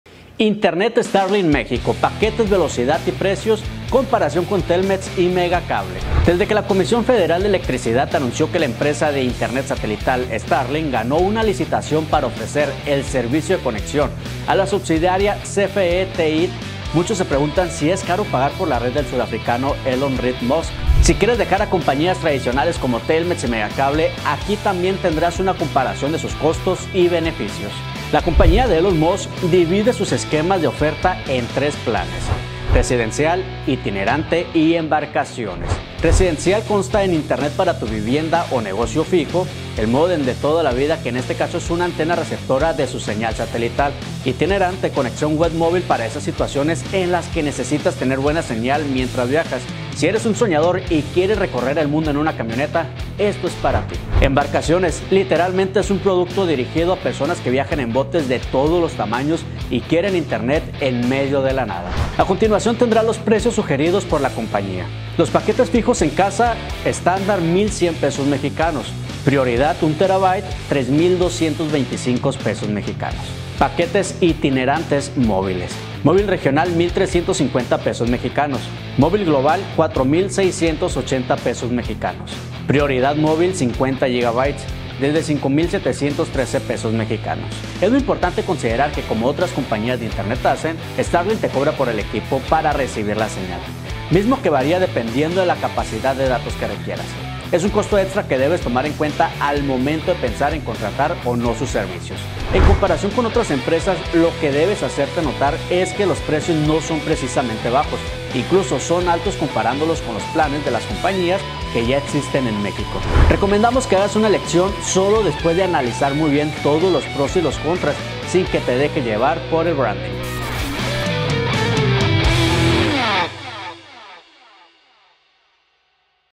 Internet Starlink México: Paquetes, velocidad y PRECIOS
Desde que la Comisión Federal de Electricidad anunció que la empresa de internet satelital Starlink ganó una licitación para ofrecer el servicio de conexión a la subsidiaria CFE TEIT muchos se preguntan si es caro pagar por la red del sudafricano Elon Reeve Musk.